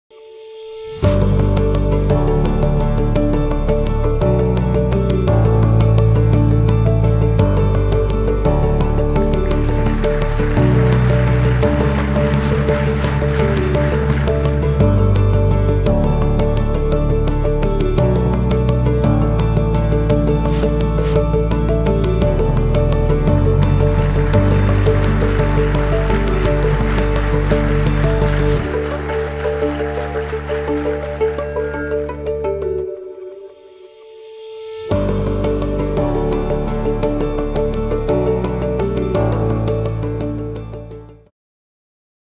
TEDxCanberra 2017 Momentum - Thankyou!
As TEDxCanberra's long term video partner we wanted to say thanks to all the speakers, volunteers and attendees that made 2017 one of the best!